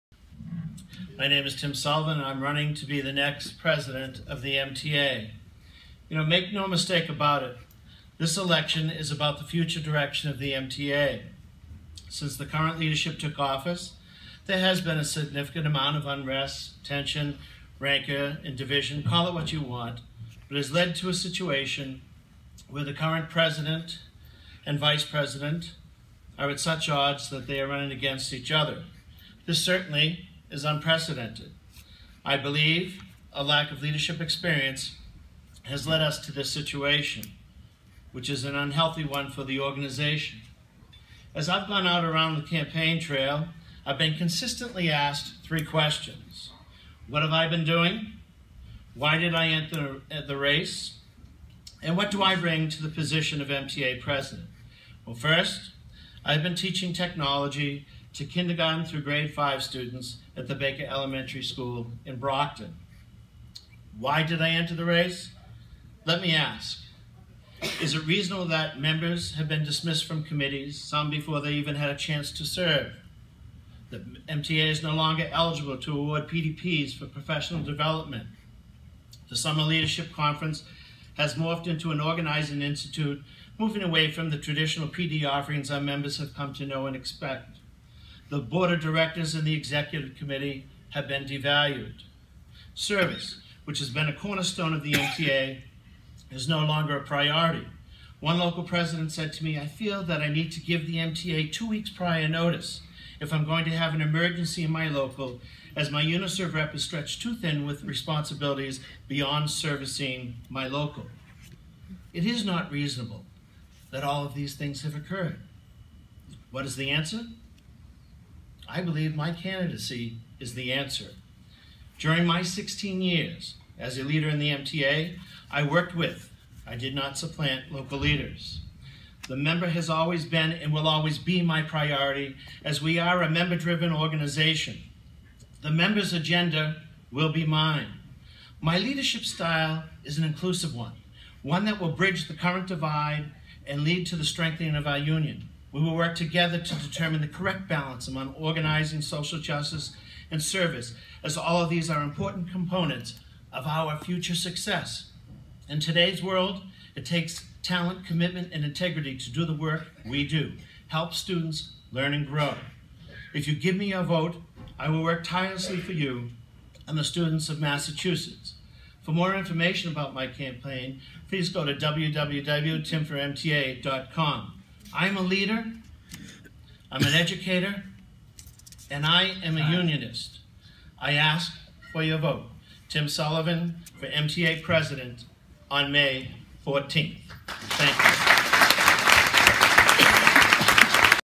Tim Sullivan, candidate for MTA president
Tim Sullivan is running for MTA president. He is the leader who will reunite the members of MTA and bring about positive change for the educators and students of Massachusetts.